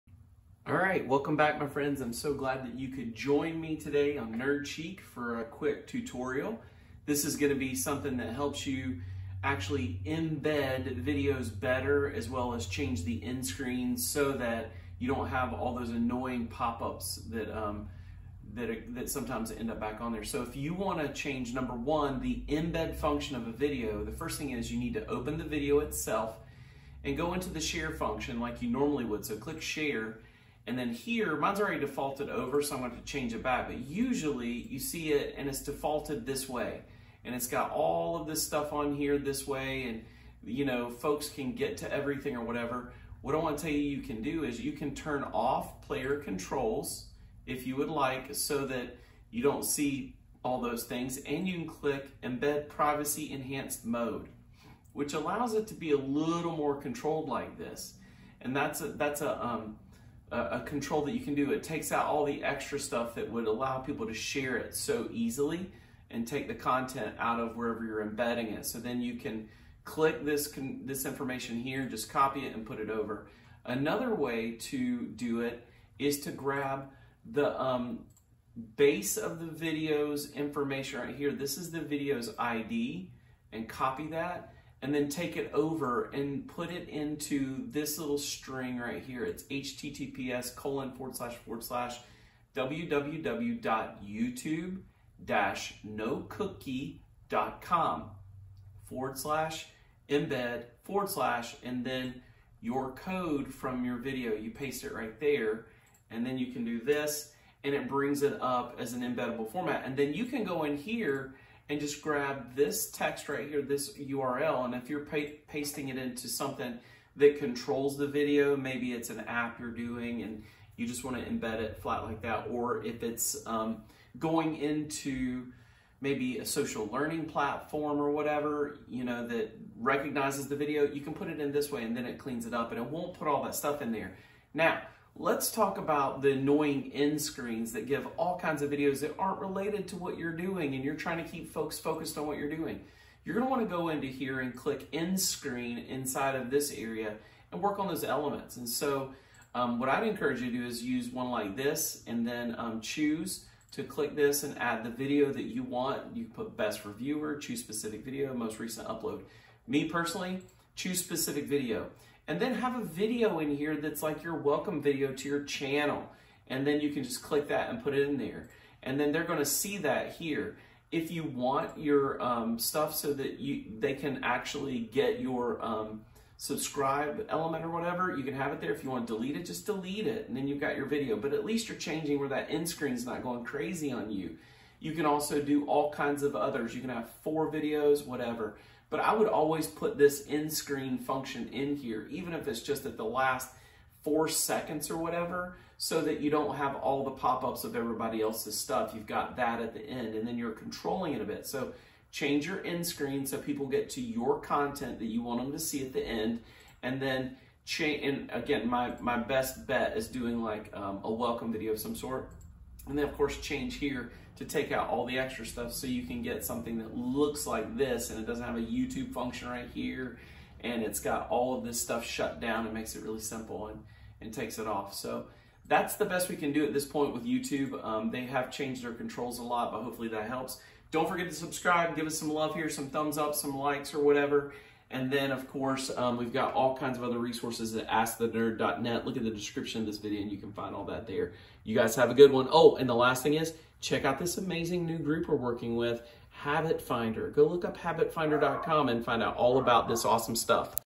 How to Control the Embed Settings & End Screen Content on a YouTube Video
Some of us really want to better manage control of our YouTube content we use in other places. The best way to do this is right here in this video. It changes all the time, but this is the quick and easy for you.

Check out the new channel I feature here. They have some incredible new content and trainings!